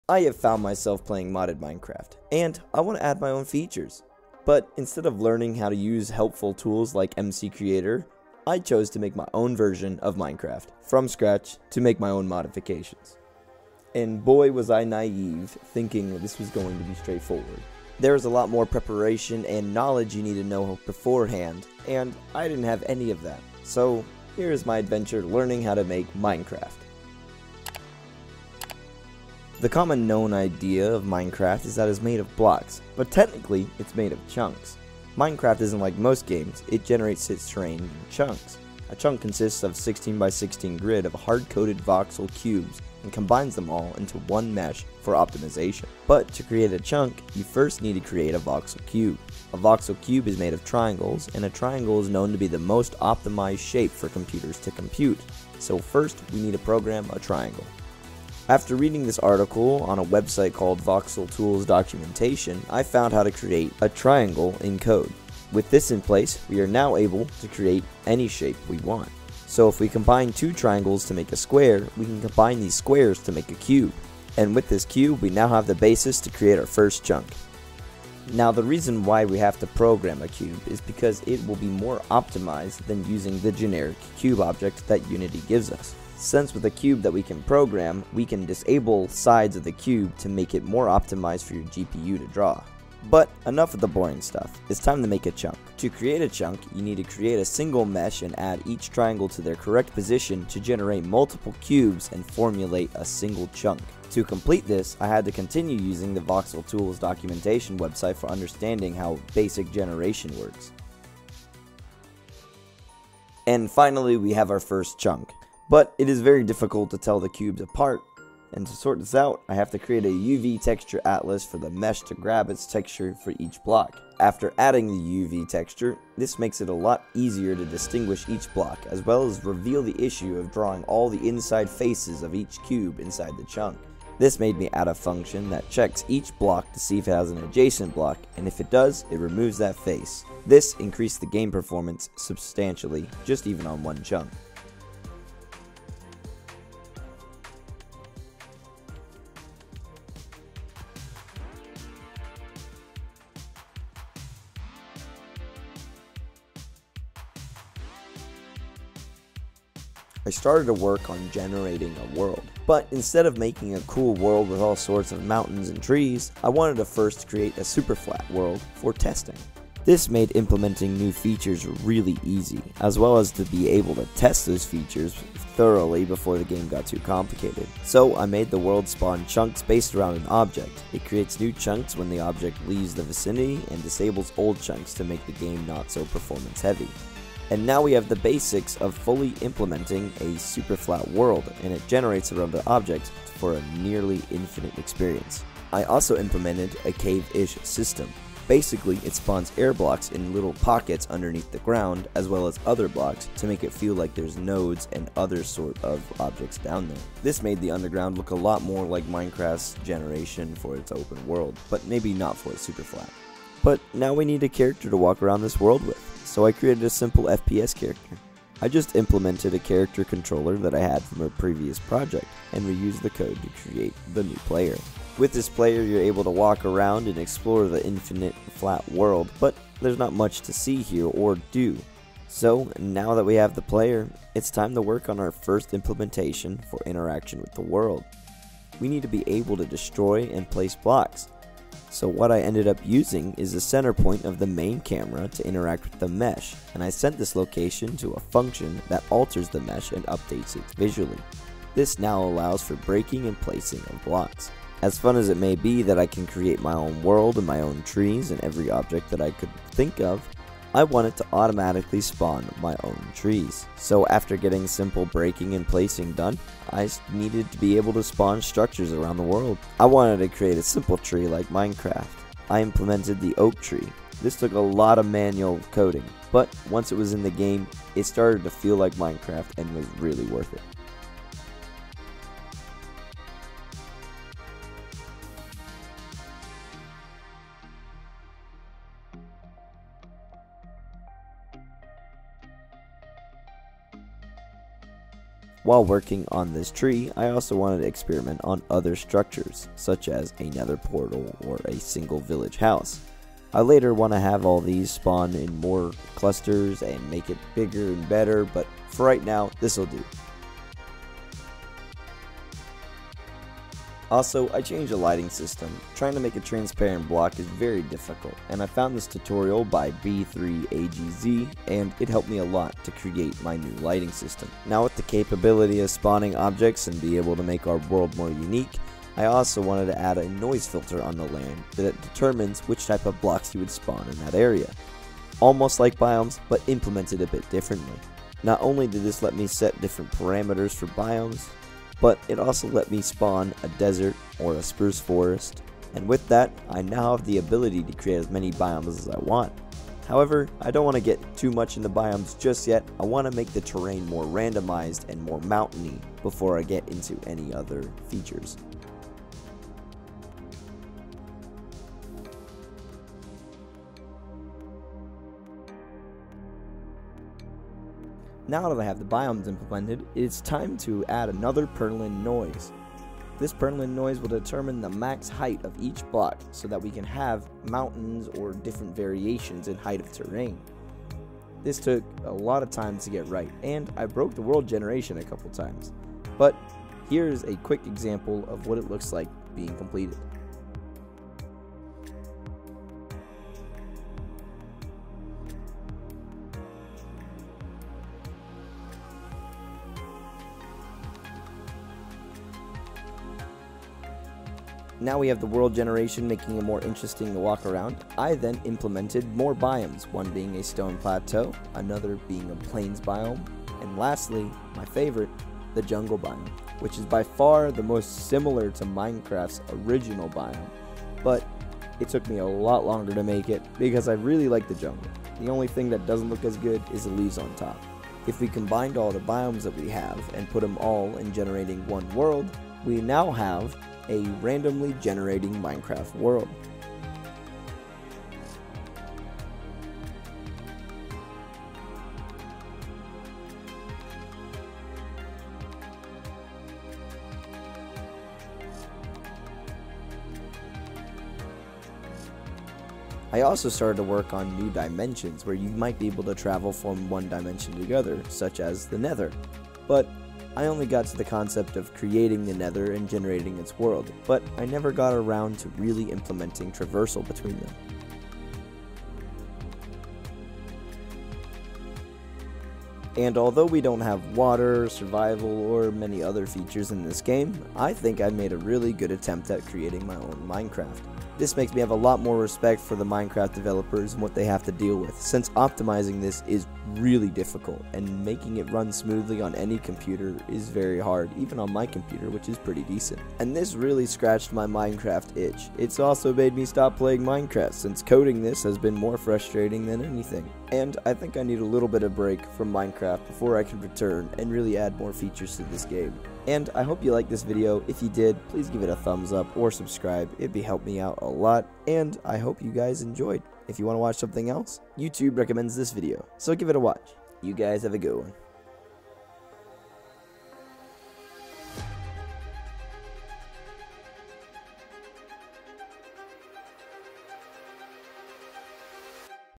Coding Minecraft from Scratch in Unity
I created Minecraft in Unity from scratch! I had to use all sorts of websites as well as online tutorials to make this. I hope you enjoy it!